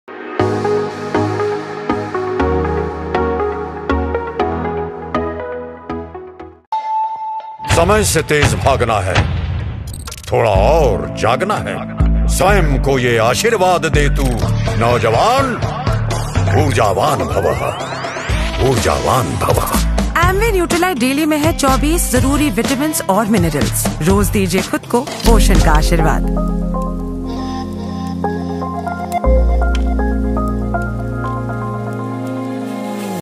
Amway Nutrilite Daily/Goodness of Multivitamin/Best Multivitamin
Amway Nutrilite Daily/Amitabh Bachchan with Nutrilite/Best Multivitamin/Nutrilite Supplements/Nutrilite Daily

About this Video:Two iconic entities, one grand purpose! Legend Amitabh Bachchan & Amway India are coming together to promote holistic health, nutrition & wellness for a progressive India, with a special focus on women and youth empowerment through guided entrepreneurship. As a part of this momentous association, we are all set to further our vision to help people live better, healthier lives.

Amitabh Bachchan - Brand Ambassador for Amway and NutriliteEvery day is the beginning of a new race and to keep winning, your body needs its dose of multiminerals and multivitamins. Like Mr.Bachchan, support yourself with 24 essential vitamins and minerals in Amway Nutrilite Daily, and give yourself an energy boost, good nutrition and immunity support! @amitabhbachchan

Nutrilite™ Daily isa convenient and effective way to support your healthy lifestyle and meet your daily nutritional requirements. With nutrients such as Vitamin C, B12, B6, folate, copper, iron and zinc; you get all the support your immune system needs. Don't wait any longer, get your daily products from Nutrilite!

Social Media Link:
1) Instagram:
     t=Q_eLmEKk1rHPFNKz-Dm4fQ&s=09
4) Linkdin: